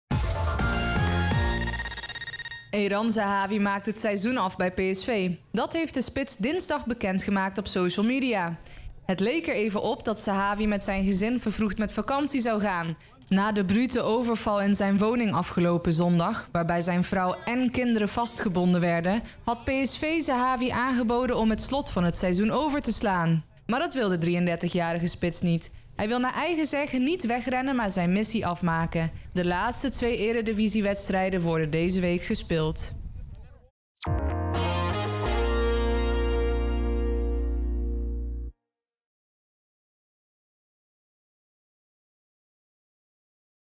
Zahavi maakt het seizoen af bij PSV
Eran Zahavi maakt het seizoen af naar PSV, dat heeft de spits bekend gemaakt op social media.